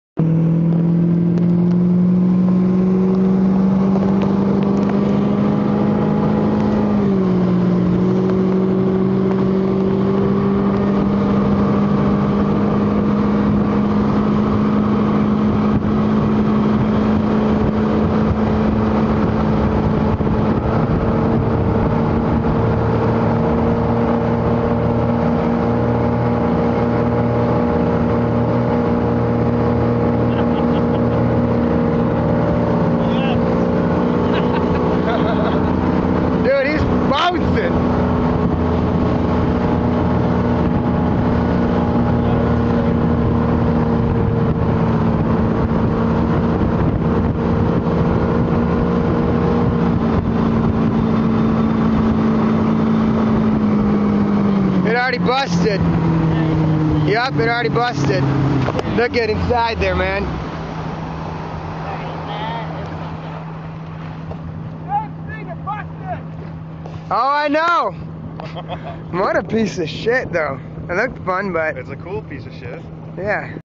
O'Brien Tube
Mike Bacon breaking my brand new O'brien tube.  He breaks everything he touches.